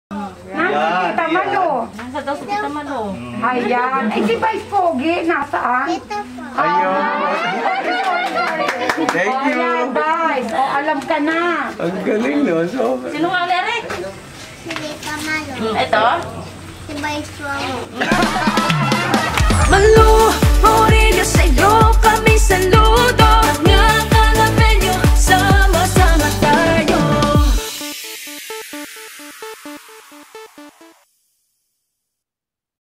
Kilala kami ni Chikiting!
Nakakatuwa talaga at kilala kami ng batang ito kahit naka-facemask kami. PusuanNaYan 💜💙